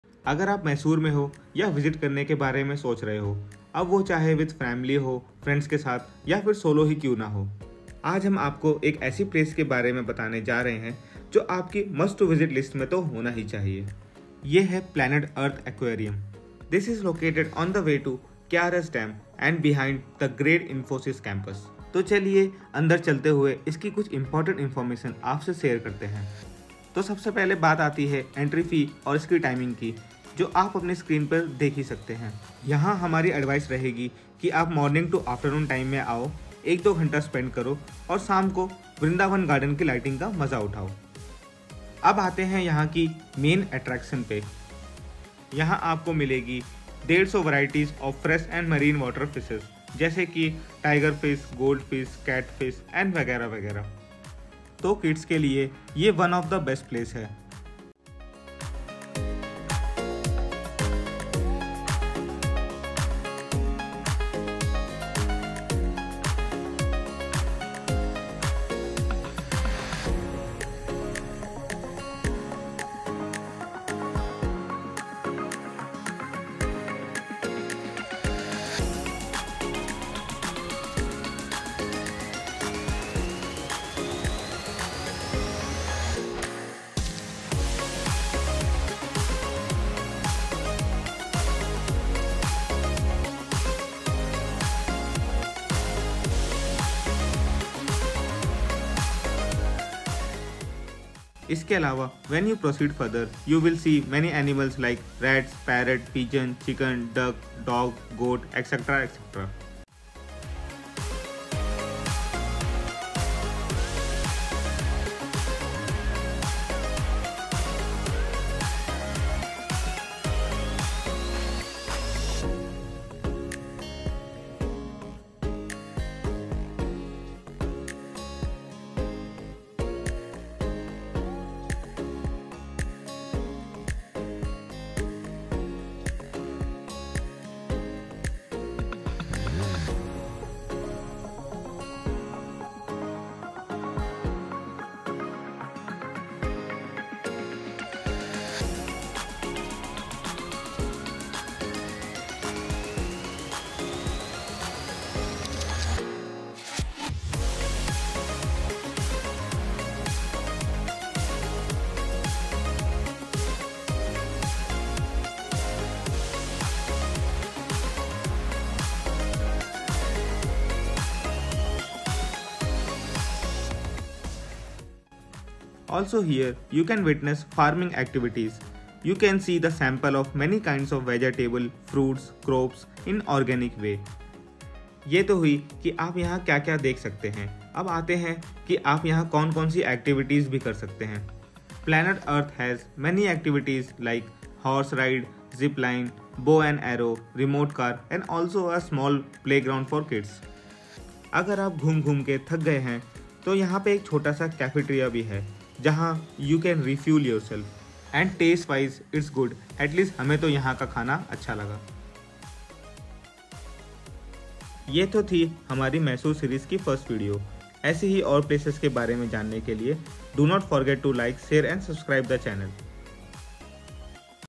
Mysore Planet Earth Aquarium | Planet Earth Aquarium & Pets Park | Places to visit in Mysore
Welcome to Planet Earth Aquarium & Pets Park, where nature meets nurture in the heart of our bustling city!

🐠 Dive into the mesmerizing world of aquatic wonders with stunning aquarium exhibits, showcasing a diverse array of colorful fish, mesmerizing corals, and exotic marine life. Whether you're a seasoned enthusiast or a curious newcomer, there's something here to captivate every imagination.

🐾 Step into Pets Park and experience the joy of animal companionship like never before! From cuddly kittens to playful puppies, furry friends are sure to steal your heart. Explore our interactive petting zoo and get up close and personal with adorable creatures big and small.

🌿 Immerse yourself in the lush greenery of our botanical gardens, where tranquility reigns supreme. Take a leisurely stroll amidst the foliage or simply relax and soak in the natural beauty that surrounds you.

🎓 Education meets entertainment at our informative exhibits and live demonstrations, where visitors of all ages can learn about conservation efforts, animal behavior, and the delicate balance of ecosystems.

🎉 Join us for a day of family fun and adventure at Planet Earth Aquarium & Pets Park. Whether you're seeking relaxation, inspiration, or simply a moment of wonder, we promise an unforgettable experience for everyone.